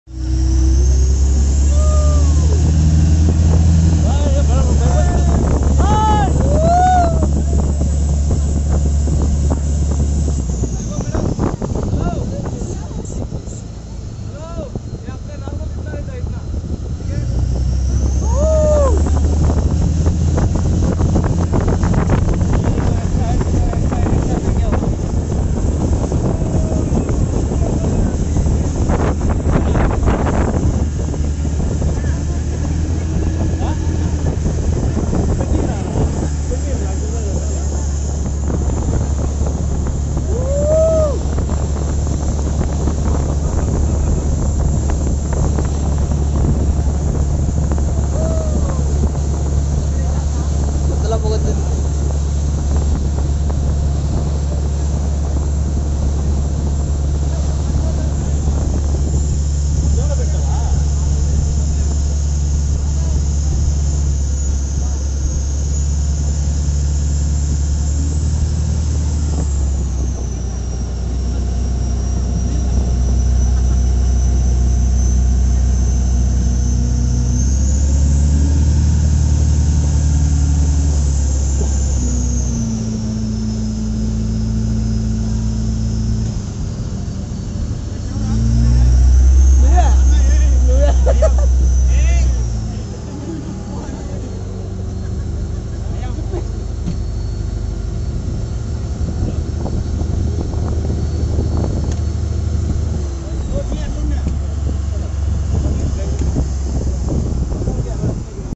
|Parasailing|Goa|Baaga Beach|
One of the most thrilling feat u need not to miss if you are heading to Goa. It costs u around 1500 including 3 dips in sea for 1.30 minutes little expensive but it was an amazing experience when u areflipping n flying in 100feet high. without dipping in sea it will cost u 1200 price is negotiable when u participate in group.